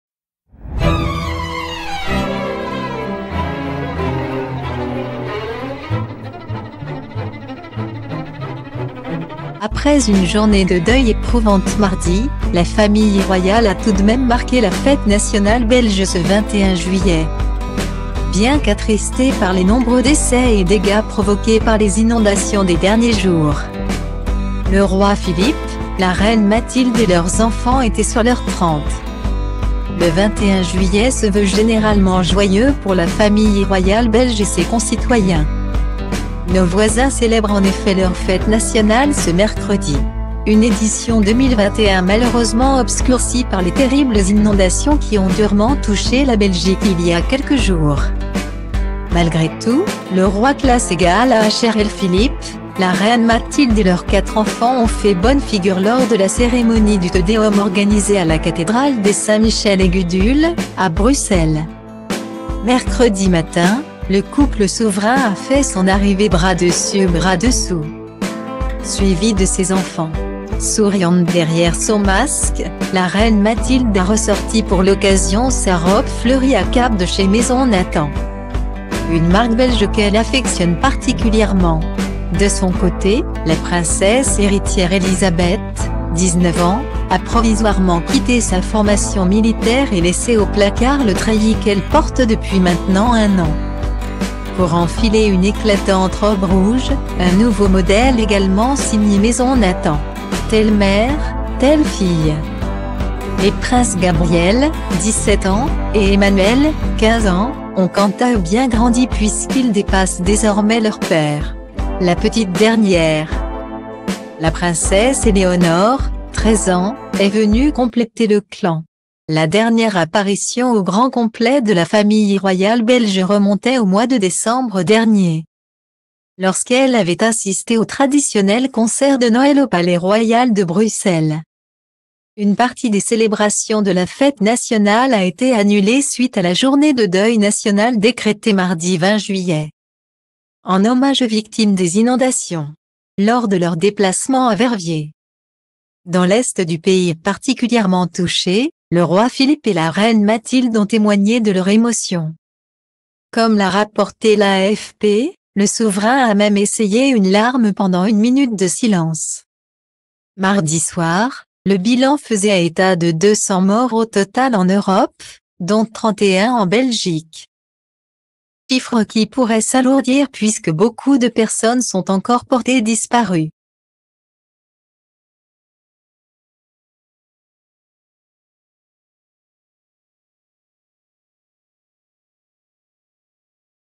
Philippe et Mathilde de Belgique en famille : Fête nationale endeuillée, le clan fait bonne figure
Après une journée de deuil éprouvante mardi, la famille royale a tout de même marqué la Fête nationale belge ce 21 juillet.Bien qu'attristés par les nombreux décès et dégâts provoqués par les inondations des derniers jours,le roi Philippe, la reine Mathilde et leurs enfants étaient sur leur trente. . .